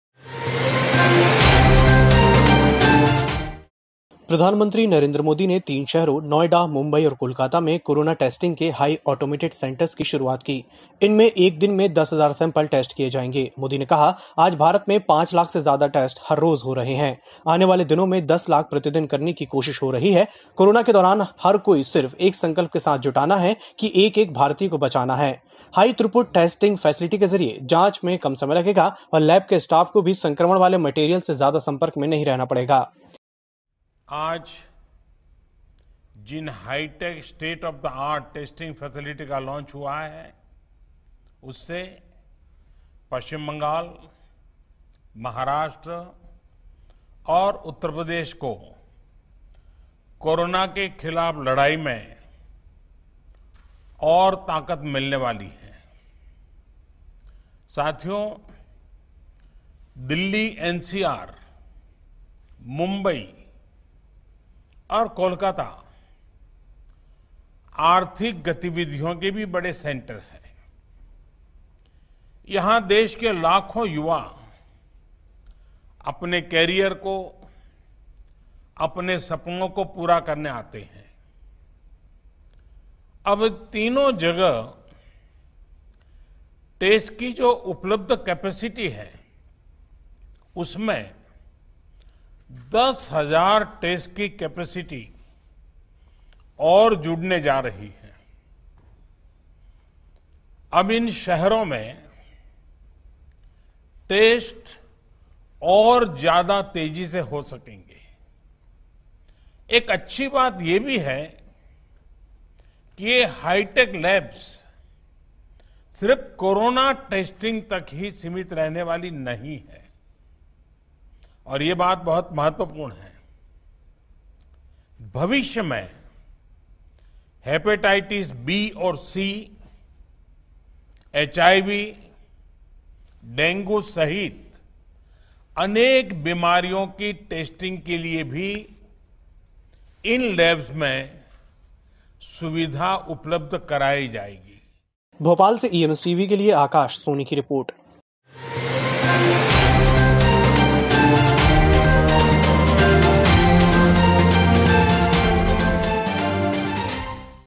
पीएम मोदी ने की कोरोना टेस्टिंग के हाई ऑटोमेटेड सेंटर्स की शुरुआत
प्रधानमंत्री नरेंद्र मोदी ने तीन शहरों- नोएडा, मुंबई और कोलकाता में कोरोना टेस्टिंग के हाई ऑटोमेटेड सेंटर्स की शुरुआत की। इनमें एक दिन में 10 हजार सैंपल टेस्ट किए जाएंगे। मोदी ने कहा- आज भारत में 5 लाख से ज्यादा टेस्ट हर रोज हो रहे हैं। आने वाले दिनों में 10 लाख प्रतिदिन करने की कोशिश हो रही है। कोरोना के दौरान हर कोई सिर्फ एक संकल्प के साथ जुटाना है कि एक-एक भारतीय को बचाना है।हाई थ्रूपुट टेस्टिंग फैसिलिटी के जरिए जांच में कम समय लगेगा और लैब के स्टाफ को भी संक्रमण वाले मेटेरियल से ज्यादा संपर्क में नहीं रहना पड़ेगा।